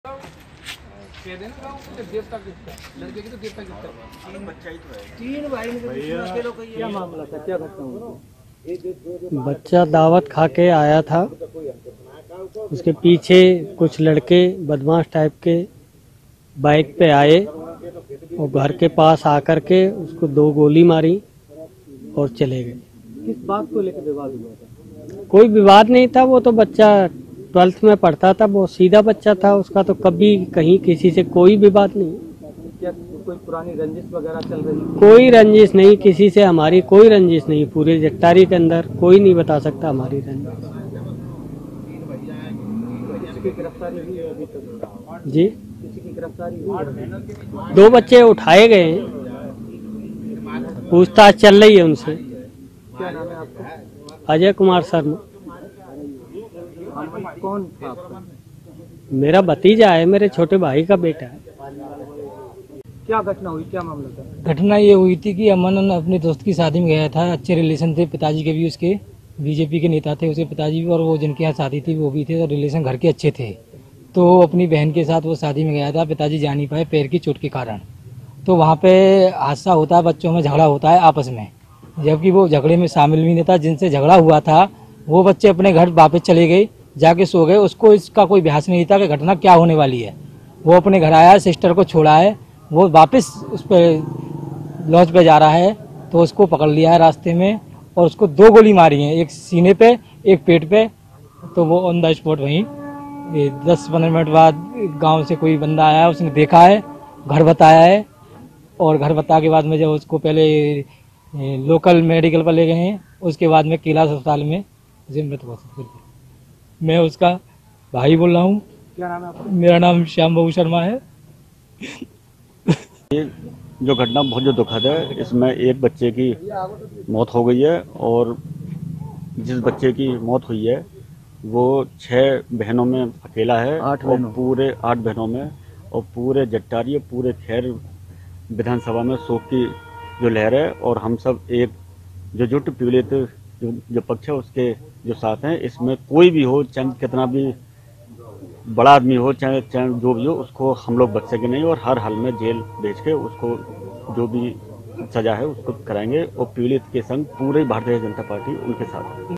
अलीगढ़: भाजपा नेता के बेटे की गोली मारकर हत्या, तीन गिरफ्तार
अलीगढ़: सोमवार देर रात बाइक सवार बदमाशों ने भाजपा नेता के बेटे की गोली मारकर हत्या कर दी. मृतक युवक नमन उर्फ ध्रुव भाजपा के टप्पल मंडल के महामंत्री विजय शर्मा का इकलौता पुत्र था. घटना की सूचना पर मौके पर पुलिस पहुंची. शव का पोस्टमार्टम कराया जा रहा है.